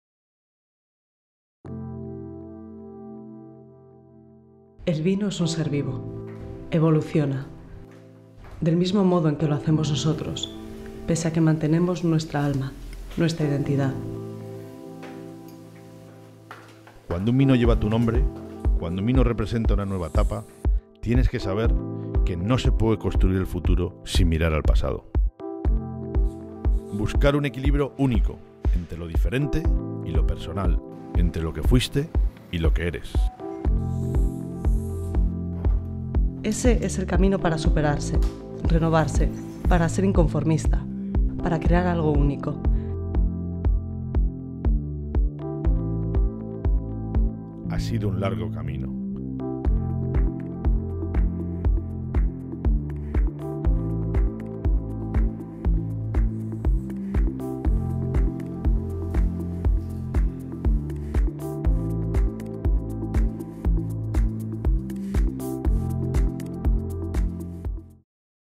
Dalmau 2011 - English Version
Dalmau 2011 - New Vintage - New Label

Wine is a living thing and evolves in the same way we do, although our soul and identity remains.

When a wine carries your own name, when a wine represents a new era, you need to know that the future cannot be created without looking back at the past.

To look for an unique balance between what is different and personal, between what you were and what you are.

That is the path to improve and to update yourself, to be maverick, to create something unique.

It has been a long journey.